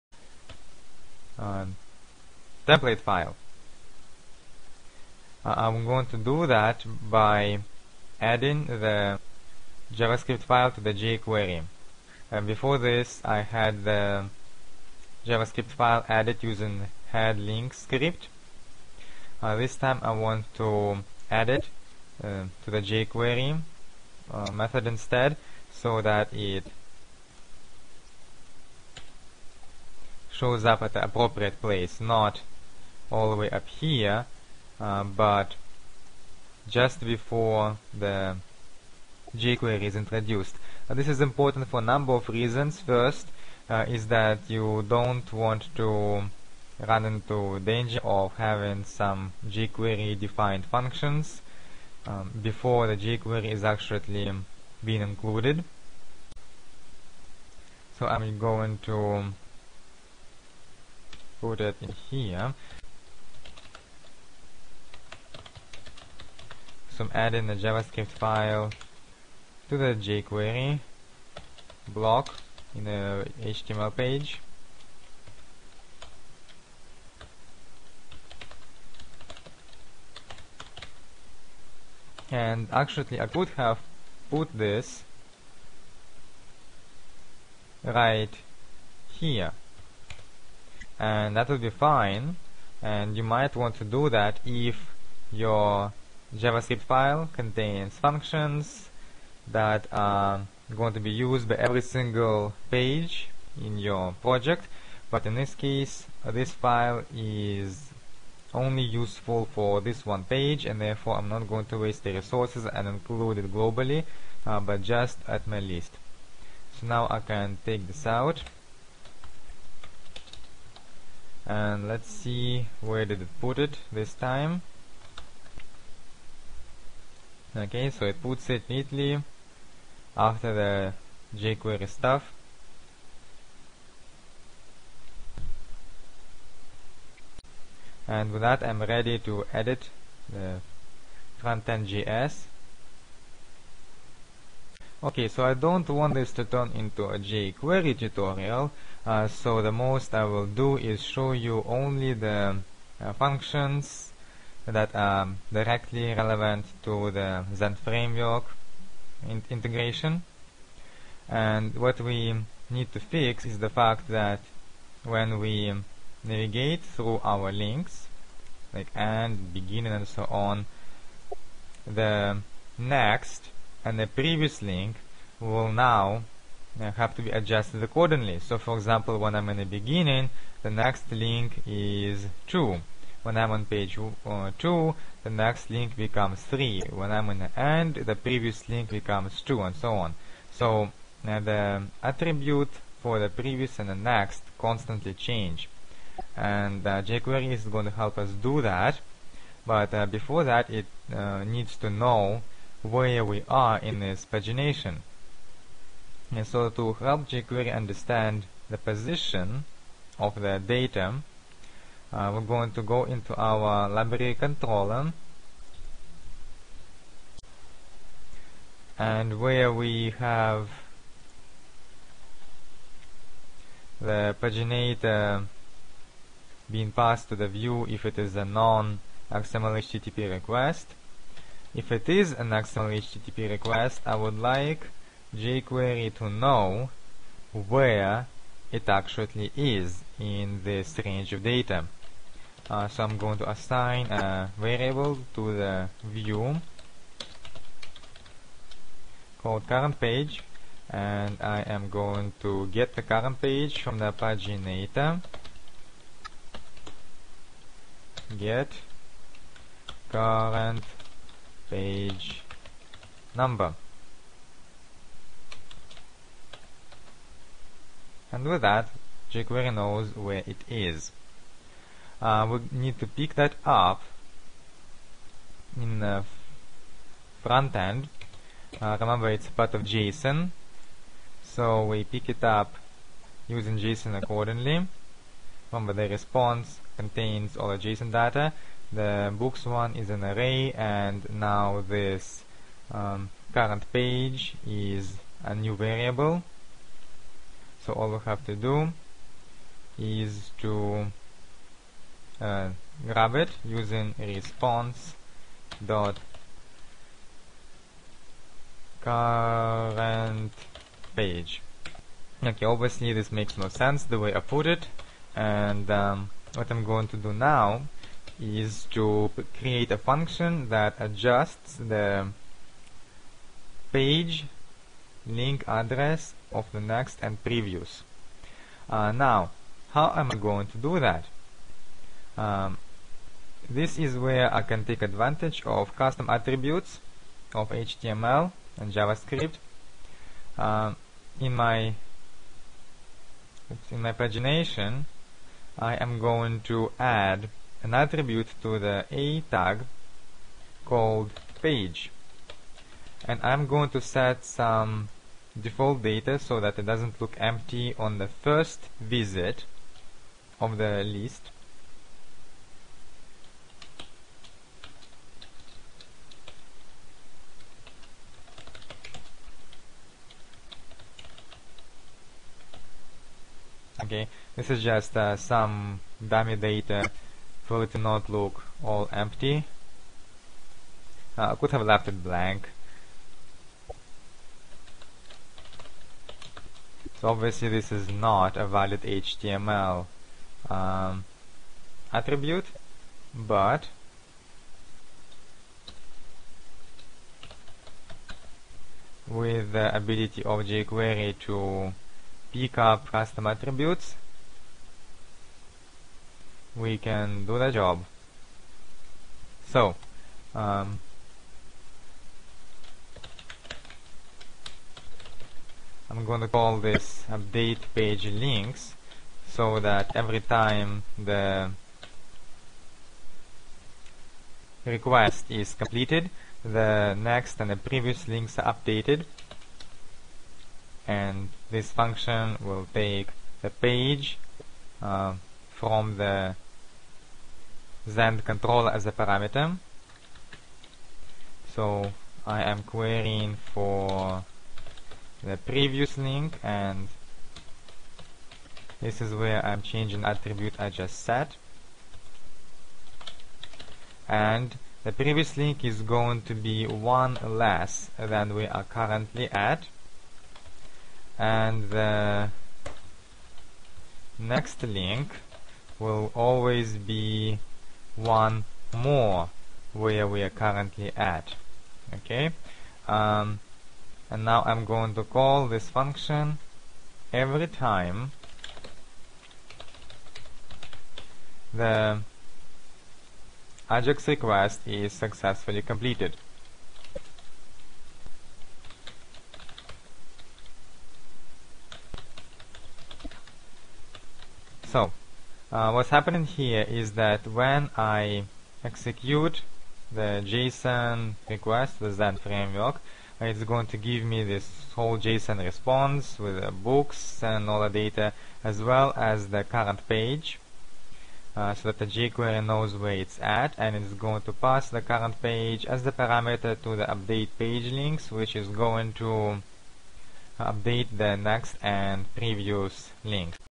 Zend Framework 1.9 tutorial 15: ZF and jQuery mix part 2
Ideas on how to mix front-end jQuery interface with back-end Zend Framework. May be boring for advanced users.